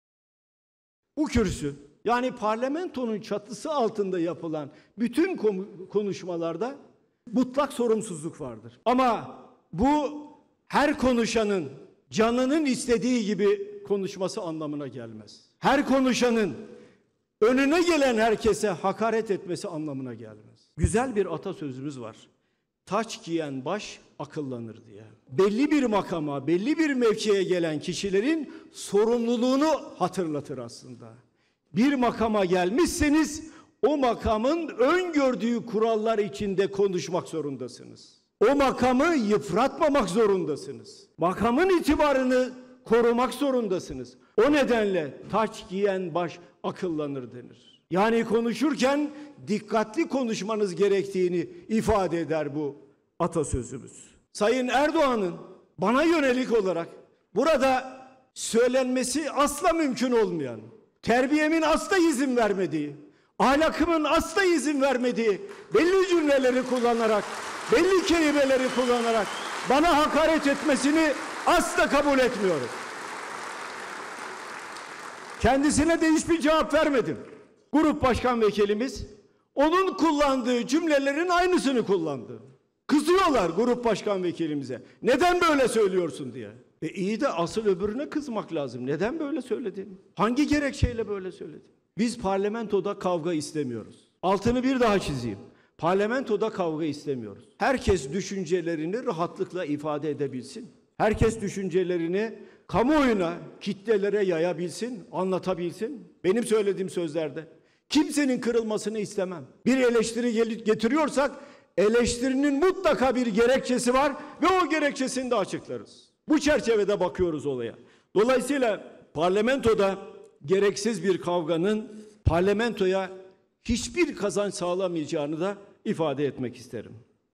Sonra da Grup Başkanvekilimiz aynı kelimeleri kullanınca...
Genel Başkanımız Kemal Kılıçdaroğlu: “Sayın Erdoğan'ın bana yönelik olarak, burada söylenmesi mümkün olmayan, terbiyemin asla izin vermediği kelimeleri kullanmasını kabul etmiyorum. Sonra da Grup Başkanvekilimiz aynı kelimeleri kullanınca...”